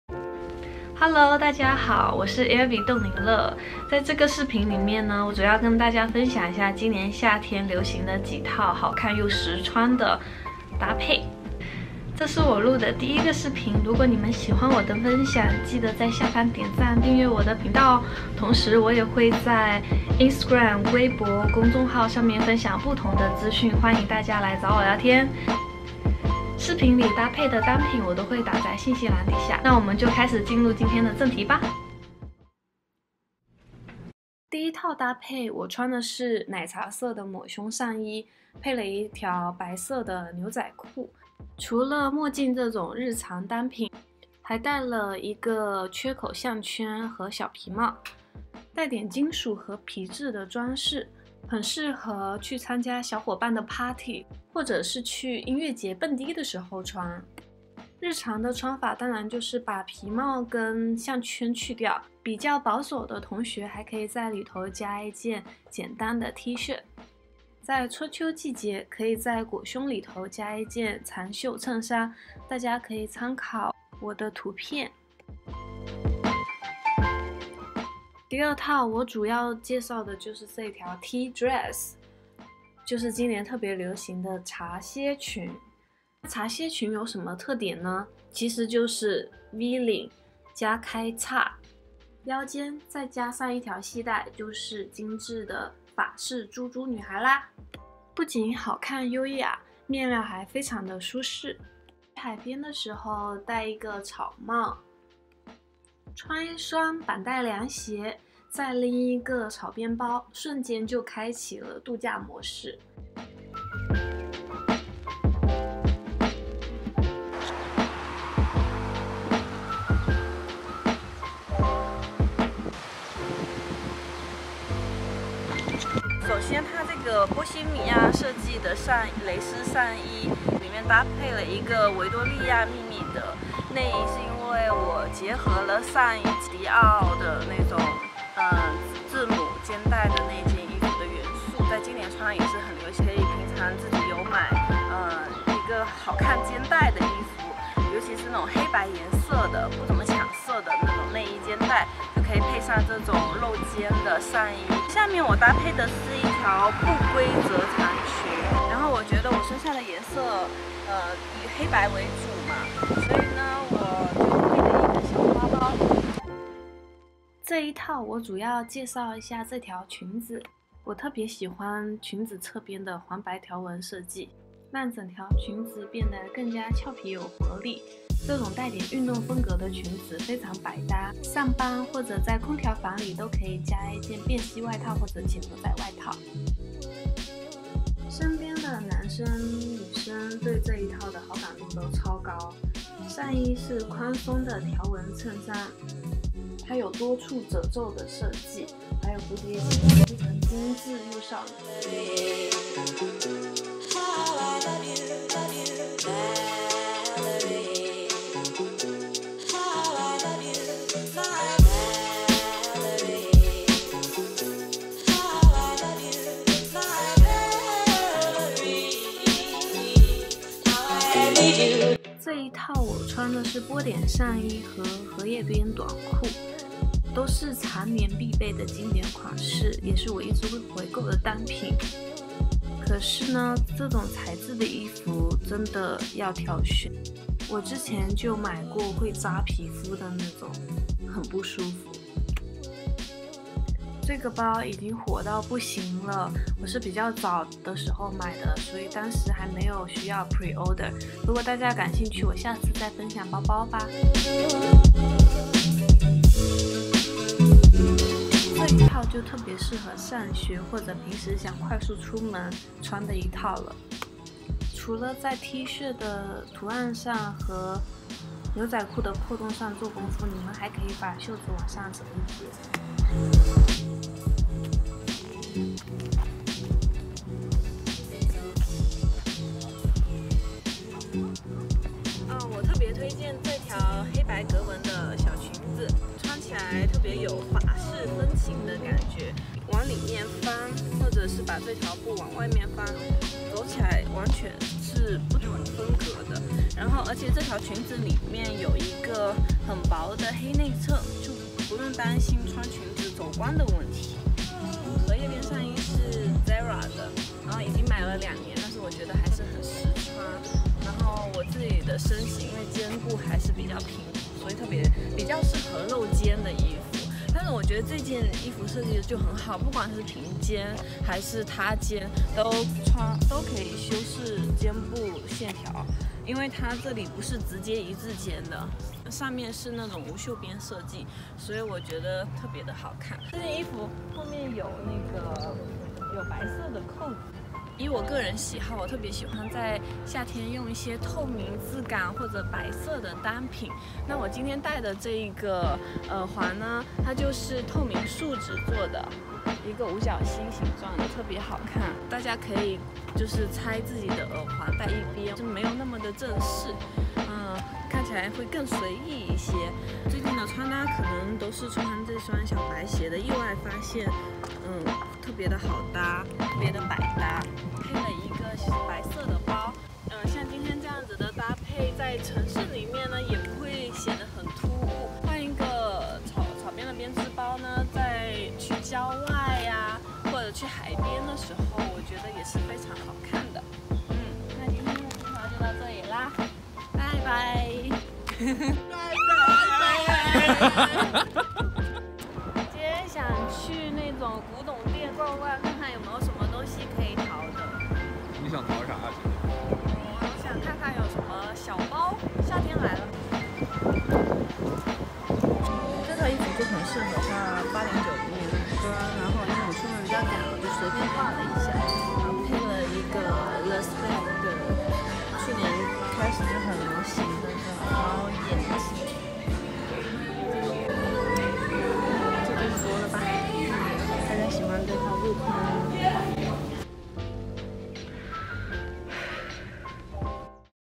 夏日穿搭合集|Summer Trends |Try-on Haul 2018 |Airbee冻柠乐
第一次出视频在剪辑方面完全不在行，大家见谅啦。
我通常体重在103kg浮动 这次视频里的我是体重最重的时期 115kg左右 所以这些衣服都还蛮遮肉显瘦的呢！（仅提供大家参考）

白牛仔短裤ZARA 🕶️：Ray Ban  皮帽：Asos

宽檐草帽 ASOS

Look 3
白色蕾丝露肩 Storets
不规则黑裙子 Storets
鞋：Converse经典小白鞋

球球绑带凉鞋 Steve Madden 水桶包 Staud

Look 5
蓝色花边蝴蝶结露肩T ASOS
白短裤 Forever21
粉色丝带草帽 ASOS

波点荷叶边短裤 Tillys店里买的（便宜又好穿）

Look 7
饮料商标T恤 ASOS 裙子MANGO 帽ASOS 包Staud

Look 8
粉帽Antisocial Club 粉色玫瑰短袖Antisocial Club
破洞牛仔 Garage

Look 9
蓝色荷叶边上衣 ZARA 不规则裙Storets 小白鞋匡威  包Parisawang

Look 10
灰色迪士尼T 复古牛仔裙Storets 拖鞋Nike

谢谢大家❤️